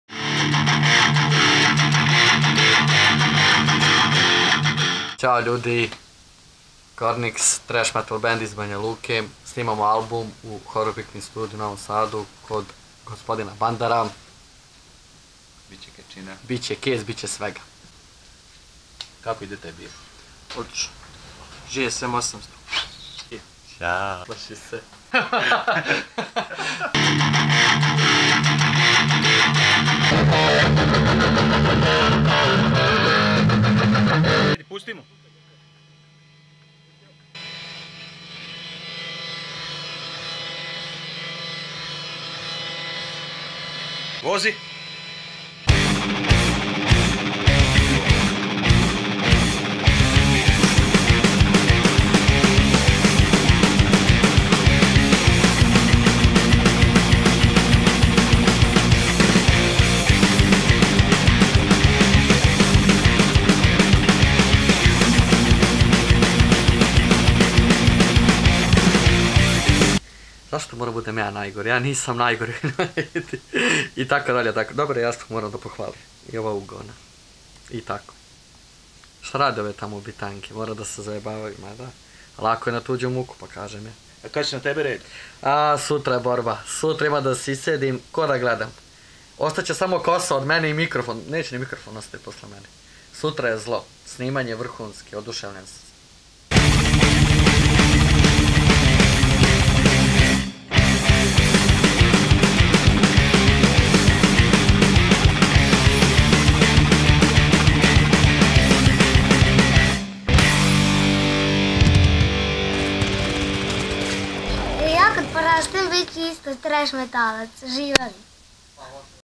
Piknik Studio - Cornix
Snimanje albuma thrash metal benda Cornix iz Banjaluke.
Piknik Studio, avgust 2017